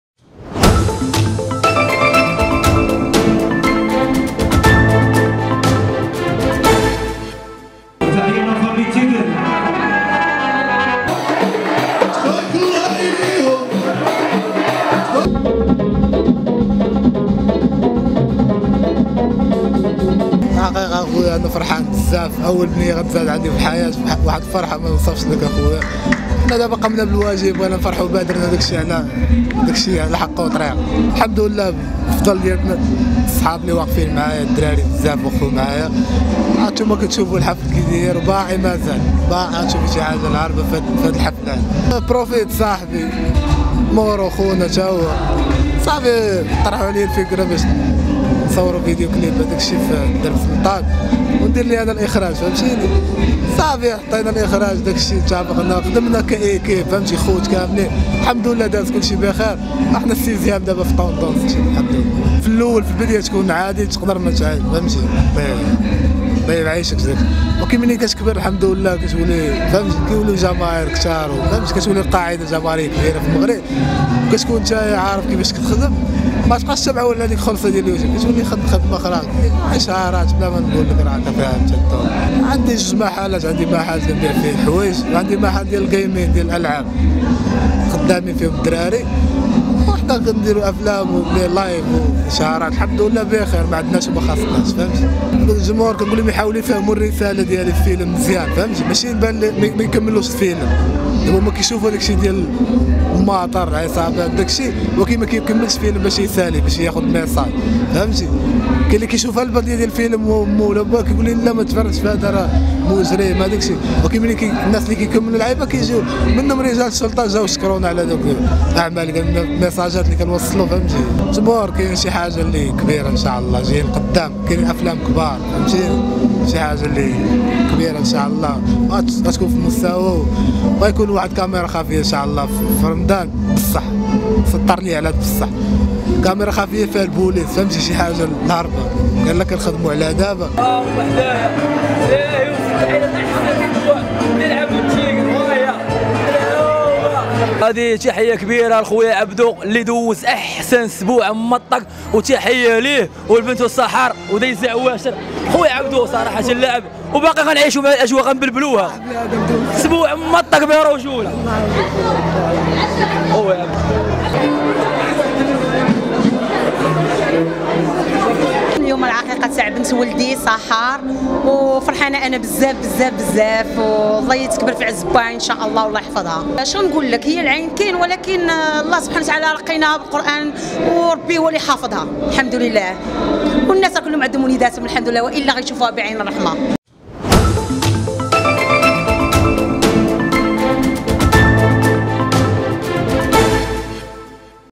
من أجواء حفل عقيقة ابنة عبدو تيغر بقصر العروس بعين السبع
موقع الكتروني مغربي، يسهر عليه نخبة منمن أجواء حفل عقيقة ابنة عبدو تيغر بقصر العروس بعين السبع الصحافيين المهنيين، يقدم الأخبار الساخنة فور وقوعها، يتم تجديده على مدار الساعة.

# أخبار_عاجلة